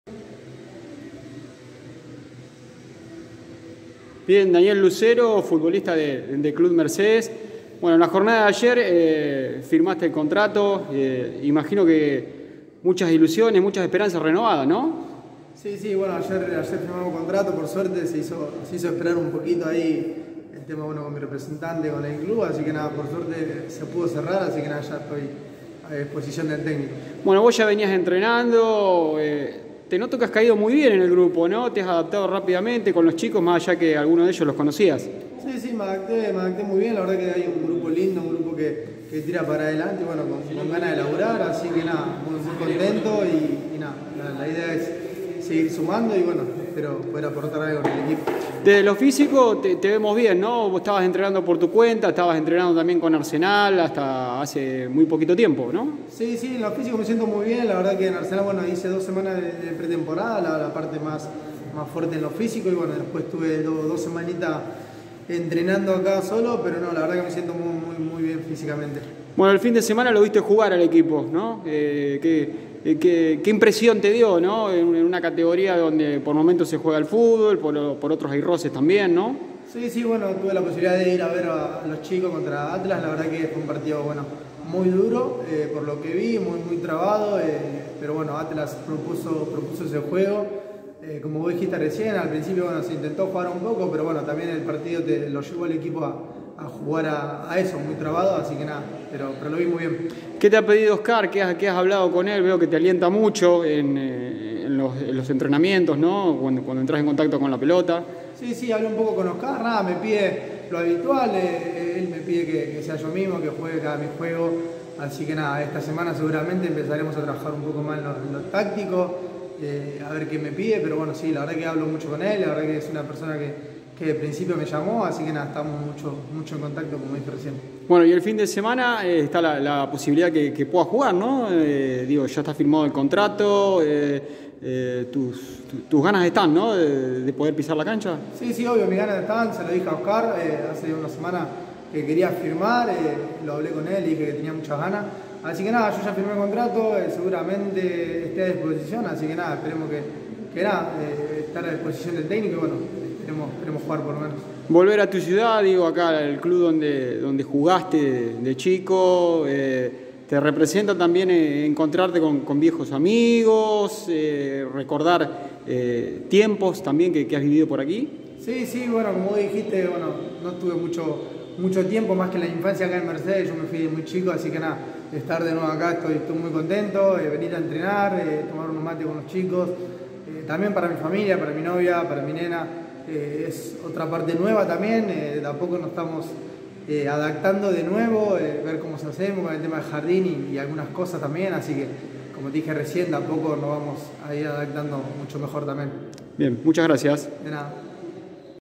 Daniel Lucero - Club Mercedes ⚽️
Refuerzo para la Primera C de la AFA proveniente de Arsenal, Macará y River.